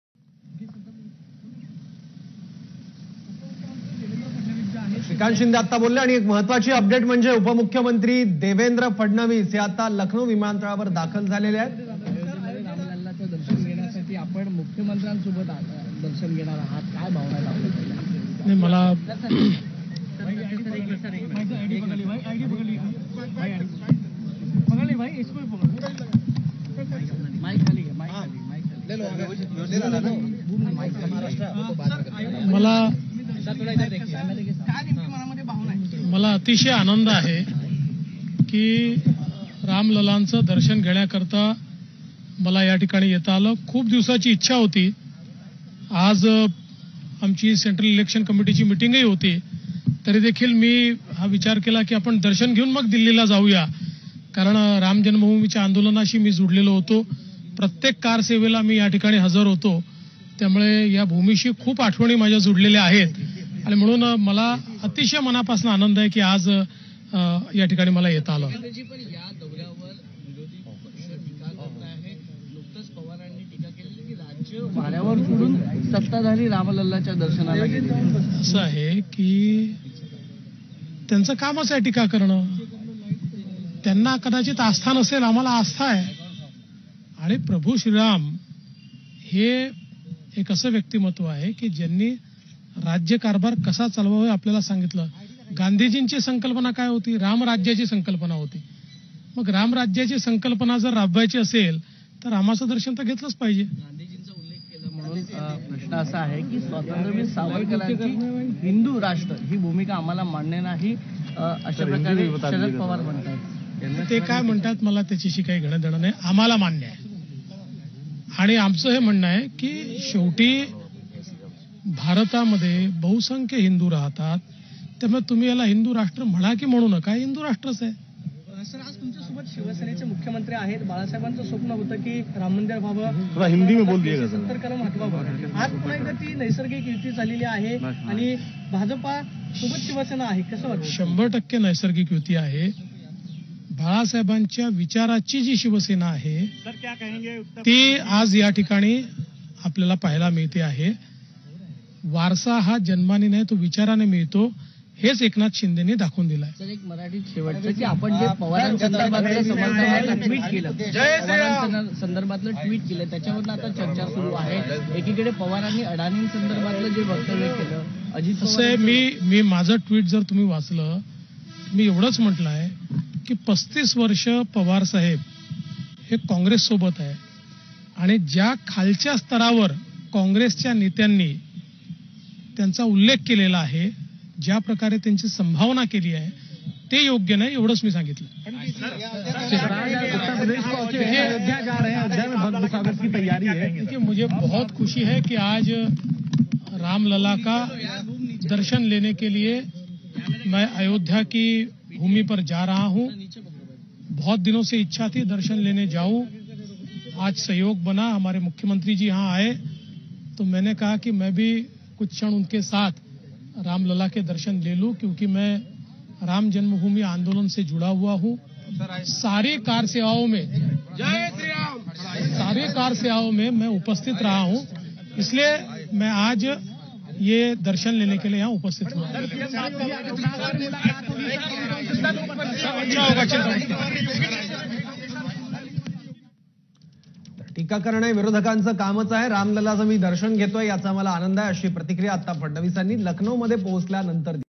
भारतामध्ये बहुसंख्य हिंदू राहतात त्यामुळे भारत 'हिंदुराष्ट्रच' | लखनऊ | DCM Devendra Fadnavis
भारतामध्ये बहुसंख्य हिंदू राहतात त्यामुळे भारत 'हिंदुराष्ट्रच' | लखनऊ | DCM Devendra Fadnavis | 9 एप्रिल 2023

0:00 - प्रभू श्री राम चंद्राचे दर्शन घेणे ही आनंदाची बाब
रामलल्लांचे दर्शन घेण्याकरता येता आले याचा आनंद. राम जन्मभूमीच्या आंदोलनाशी कारसेवक म्हणून मा. उपमुख्यमंत्री देवेंद्र फडणवीस यांचे नाते. अयोध्येच्या भूमीशी त्यांच्या खूप आठवणी.

01:08 - विरोधकांचे काम आहे टीका करणे
विरोधकांना प्रभू श्रीराम यांच्याबाबत कदाचित आस्था नसेल, महायुती सरकारला आहे. प्रभू श्रीराम हे असे व्यक्तिमत्व आहे, ज्यांनी राज्यकारभार कसा चालवावा हे सांगितले. गांधीजींची रामराज्याची संकल्पना राबवायची असेल तर रामाचे दर्शन घ्यावेच लागेल.

01:47 - भाजपाला वीर सावरकरांची भूमिका मान्य!
भारतामध्ये बहुसंख्य हिंदू राहतात, त्यामुळे हे हिंदू राष्ट्रच आहे!

02:09 - भाजप-शिवसेना ही 100% नैसर्गिक युती!
हिंदुहृदयसम्राट बाळासाहेब ठाकरे यांच्या विचारांची जी शिवसेना आहे, ती मुख्यमंत्री एकनाथ शिंदे यांच्याकडे आहे. वारसा हा जन्माने नाही तर विचारांनी मिळतो, हेच त्यांनी दाखवून दिले.

02:37 - मा. शरद पवारसाहेब 35 वर्षे काँग्रेससोबत आहेत, ज्या खालच्या स्तरावर काँग्रेस नेत्यांनी त्यांचा उल्लेख केला, ते योग्य नाही!

03:10 - अयोध्या जाकर रामलला का दर्शन करना बहुत खुशी की बात
रामलला के दर्शन करने की बहुत दिनों से इच्छा थी। राम जन्मभूमि आंदोलन से जुड़ा हुआ हूँ। सारी कारसेवाओं में हिस्सा लिया।